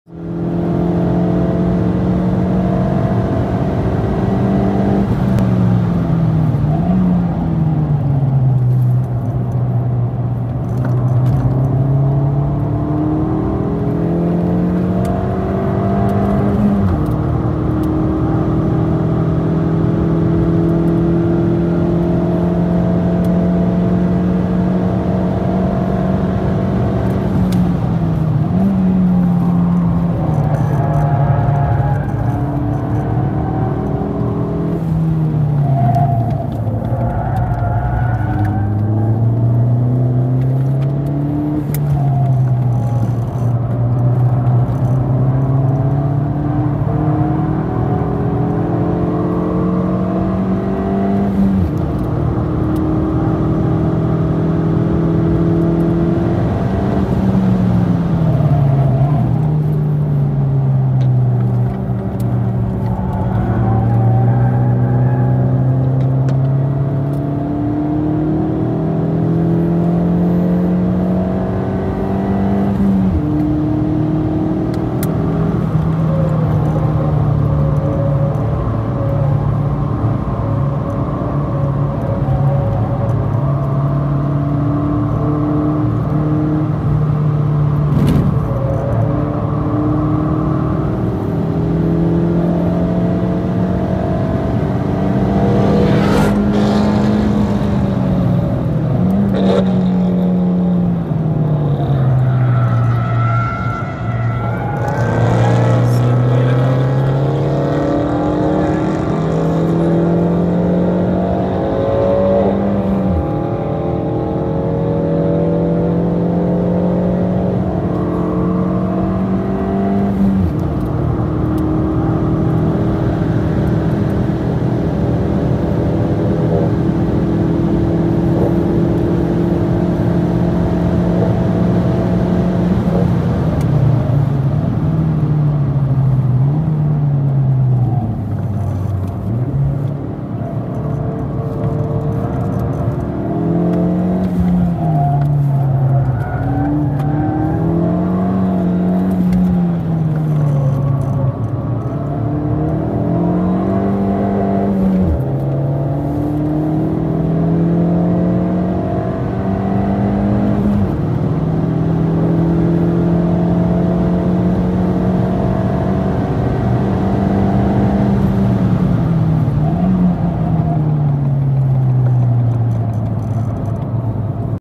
Fiesta ST in F1 Track | Istanbul Park Track Day | 2.42.9
My first Istanbul Park track experience with Fiesta ST.

Car: Fiesta ST MK7 180hp (2016)
Tires: Michelin Pilot Sport 4 - 205/40/17
Extras: K&N Panel Air Filter, Resonator delete
Everything else is stock.

Some audio parts in the video are edited to delete dialogs.